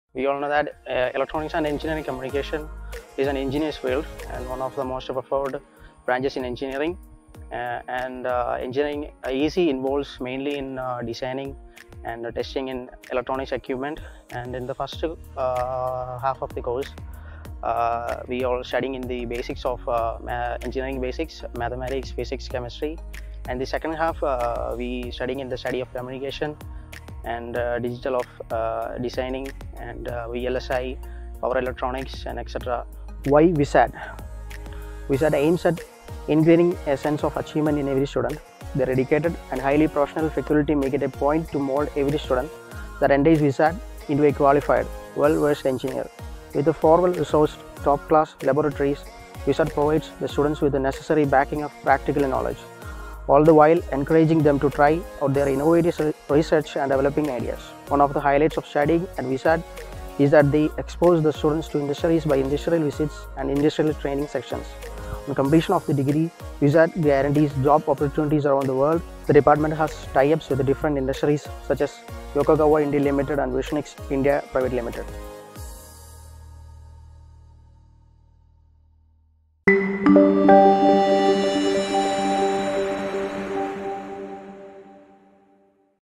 Electronics and Communication Engineering 2021-22 Admission Started at VISAT
Admission Open
2021-22

The ECE department at VISAT is highly advanced and has state-of-the-art facilities, catering to the rapid developments in the field of electronics and communications. The department is equipped with full fledged and top-notch laboratories and the best faculty. Practical learning is promoted and students are prepared to face the challenges of the future.

For course and admission details,